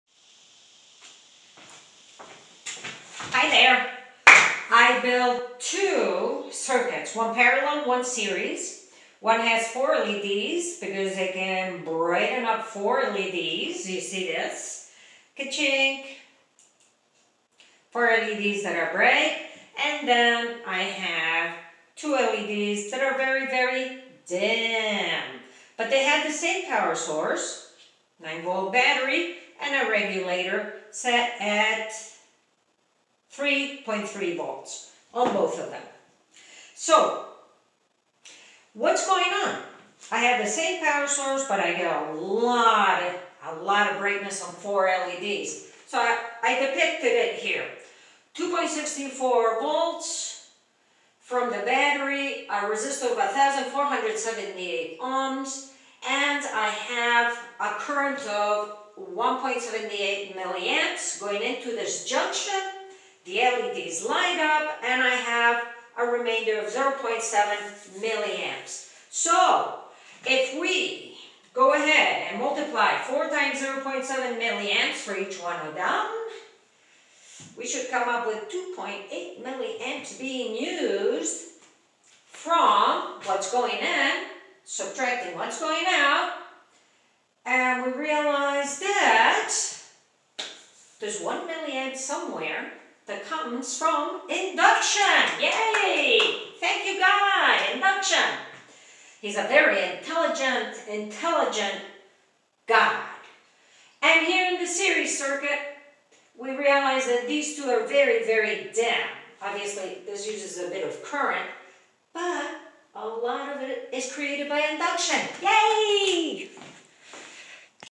Induction - Thank you God.
Two circuits are compared, one parallel with four LEDs and one series with only two LEDs.  Induction is deducted.  God has made one of his intelligent treasures seen.  Thank you God.